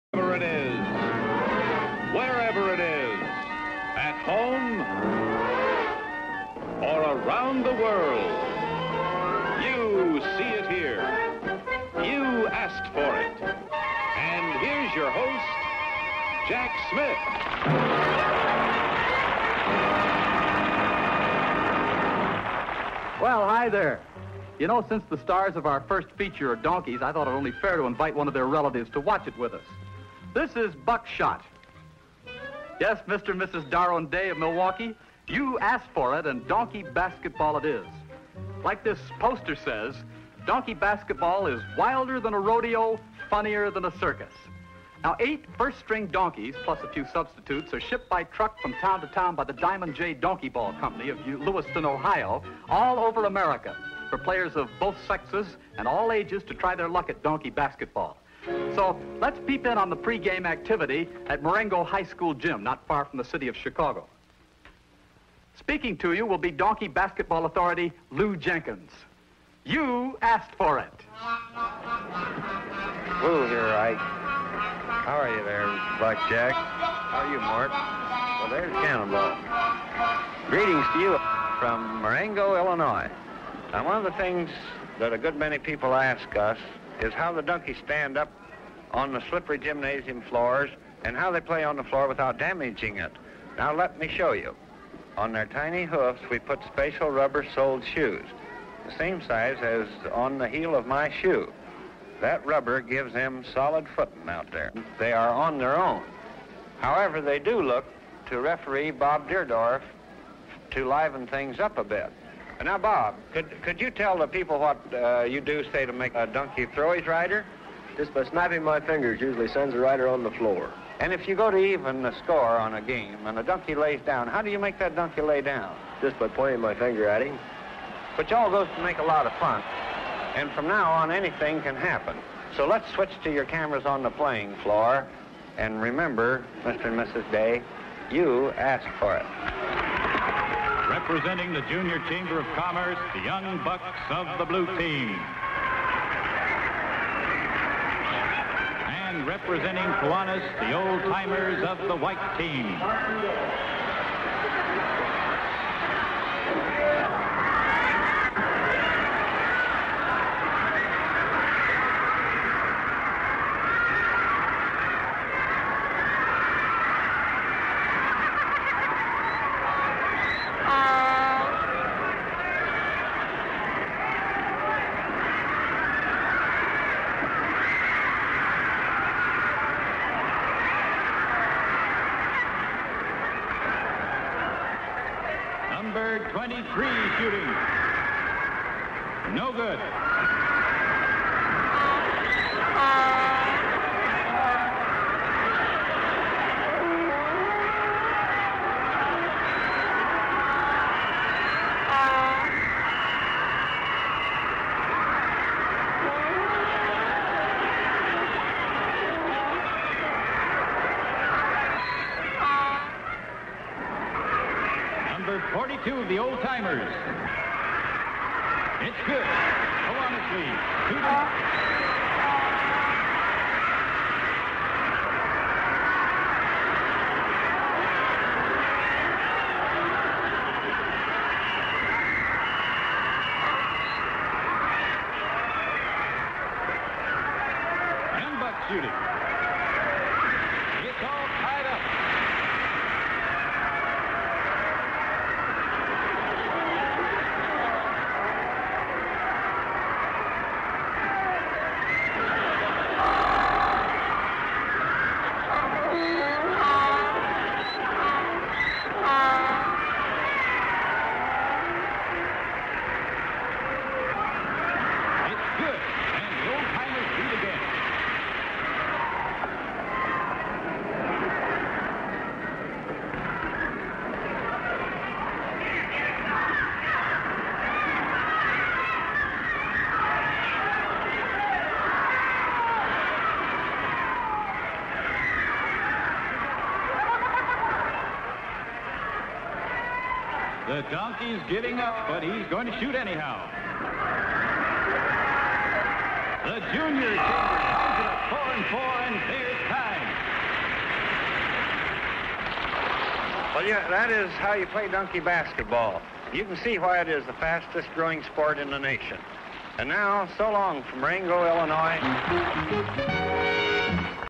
Donkey Basketball: The Young Bucks vs. The Old Timers
In Morengo, Illinois, Moregno High School hosts basketball games. But the kicker, is the players are accompanied by donkeys on the court! Watch here to see the four-legged athletes play!